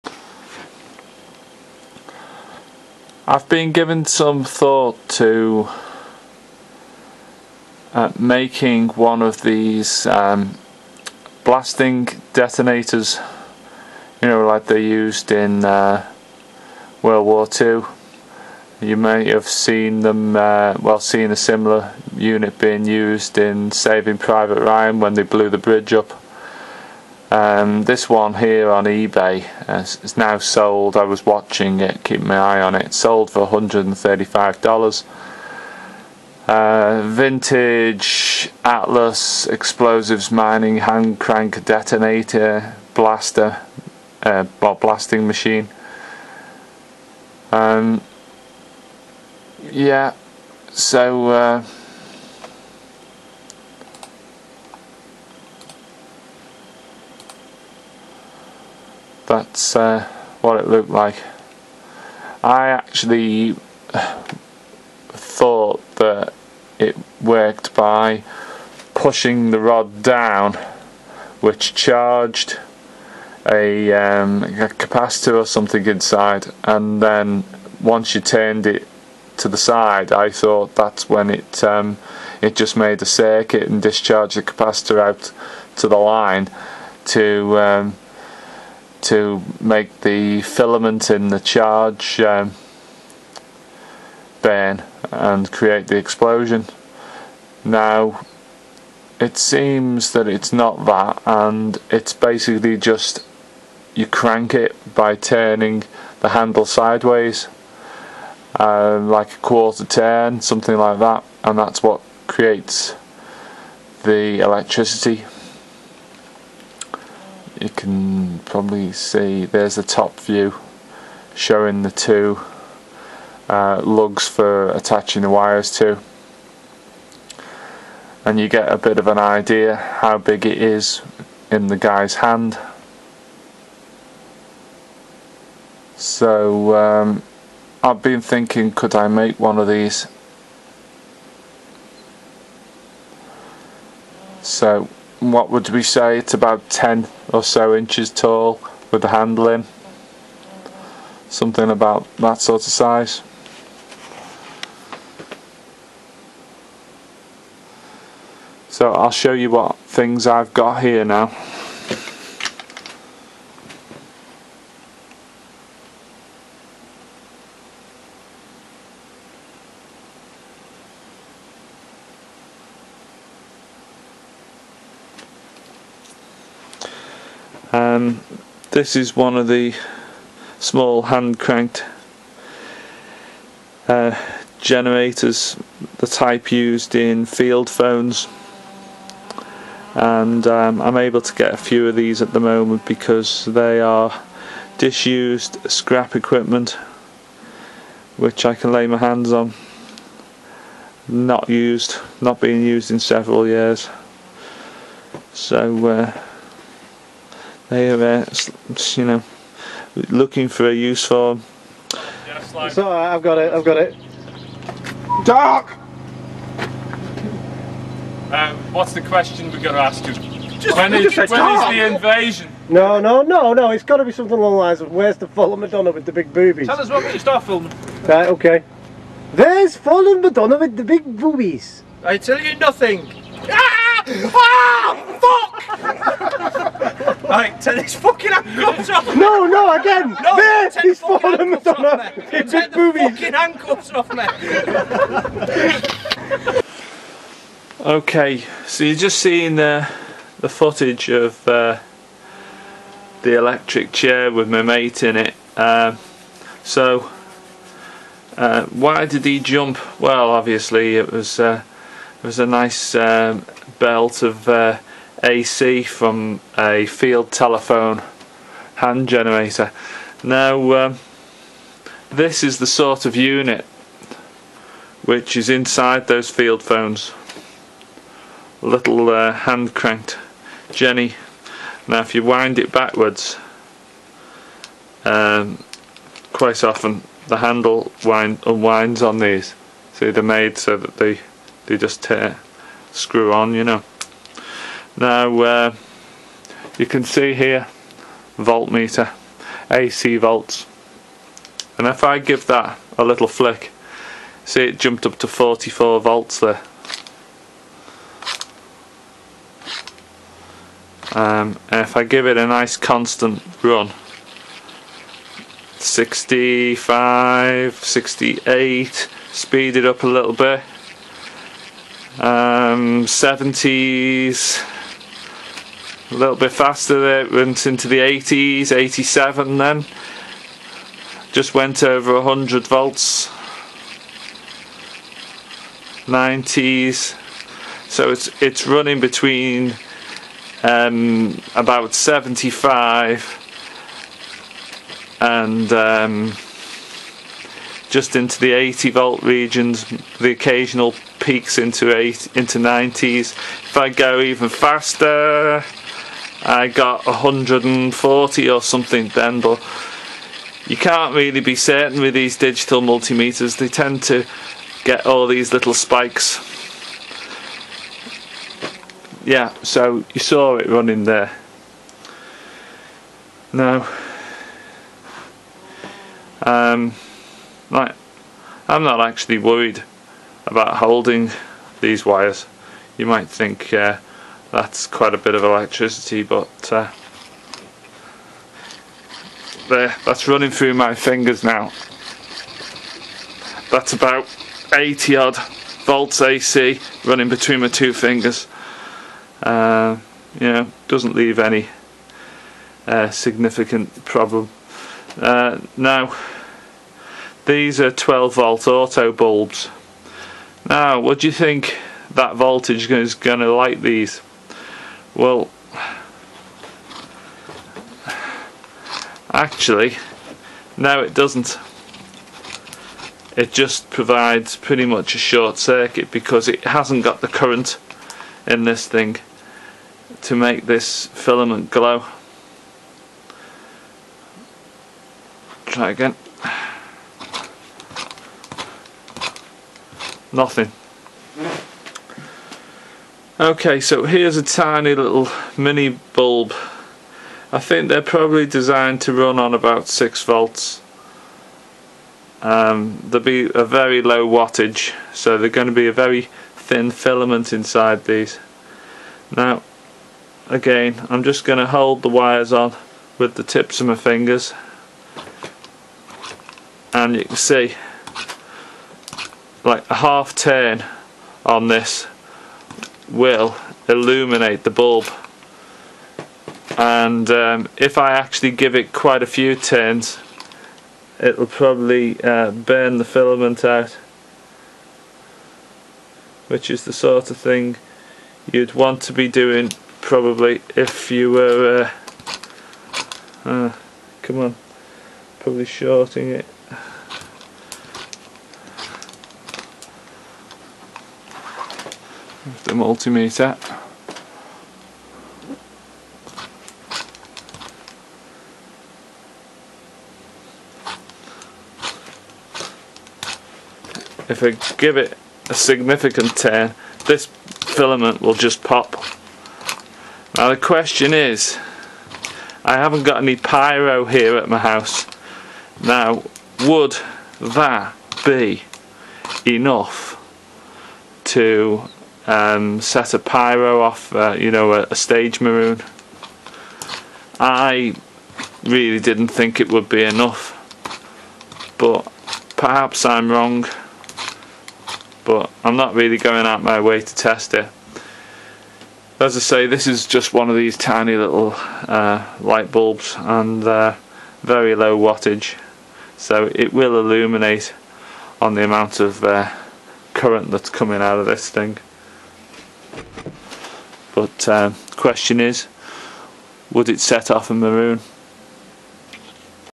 Blasting detonator project 1
Some thoughts on making a blasting detonator.